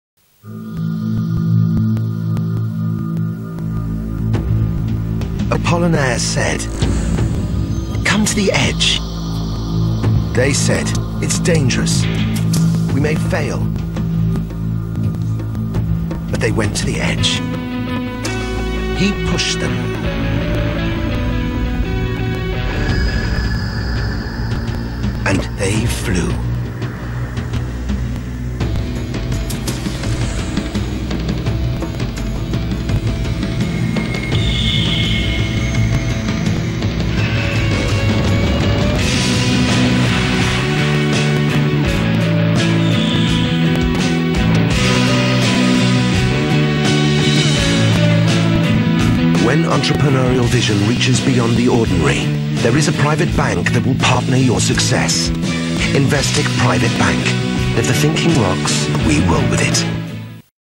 investec Private Banking
A man swings through a waterfall after absailing down.